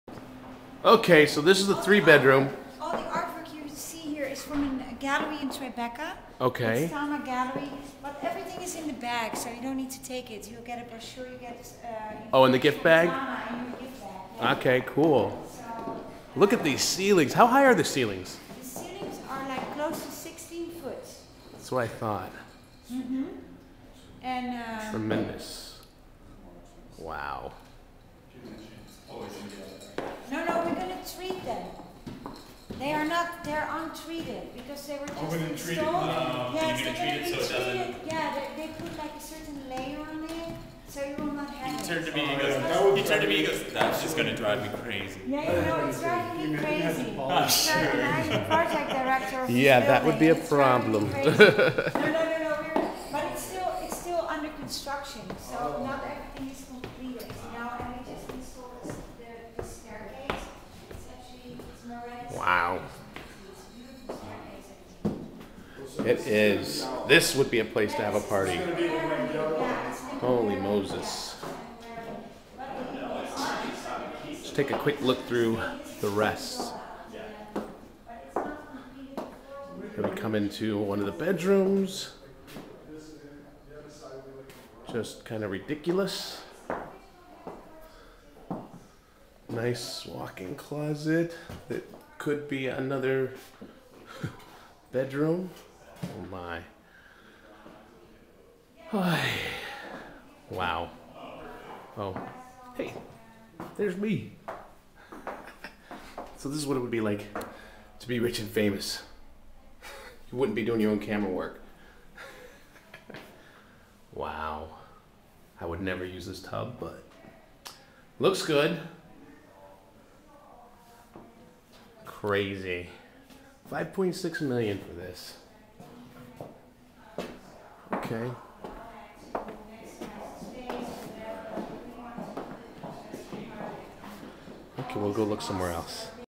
DCTOURPT3.AVI
Mike Woods finishes that fancy condo tour